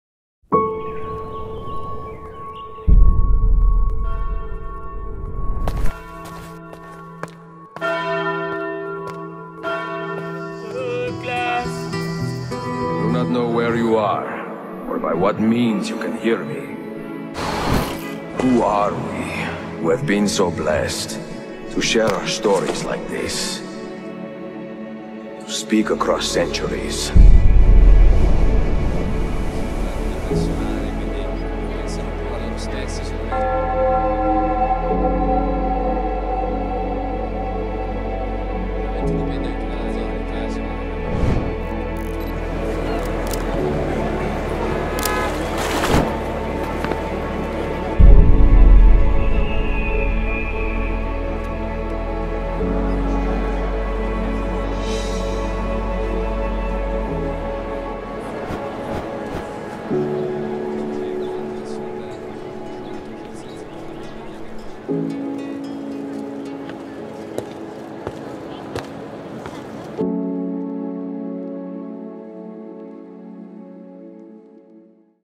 Italy, Rome 2022 - Cinematic Travel Video (Sony A7iii)
This one is for the Assassin's Creed lovers... A quick highlight of my trip to Rome earlier this year. I went for my birthday (July) and it was quite a challenge filming all the ancient buildings under the intense heat. I took my new Ronin SC2 with me, and you can clearly see the levels this gimbal sets in terms of smooth filmmaking for solo creators like myself.

Equipment:
   - SonyA7iii